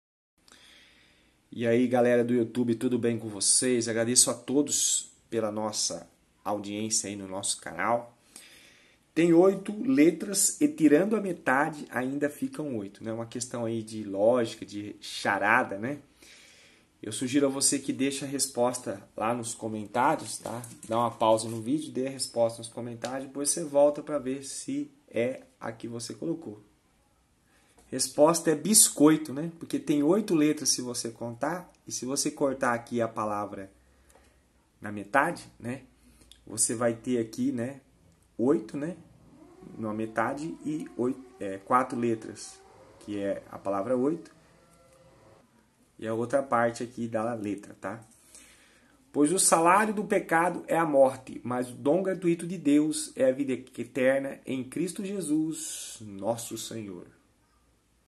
1 CHARADA TOP DAS GALÁXIAS #202 - DESAFIO DE LÓGICA - O QUE É O QUE?
Tem oito letras e tirando metade ainda ficam oito?

PLAYLIST DO CANAL  QUESTÕES DE CONCURSO (OUTRO CANAL)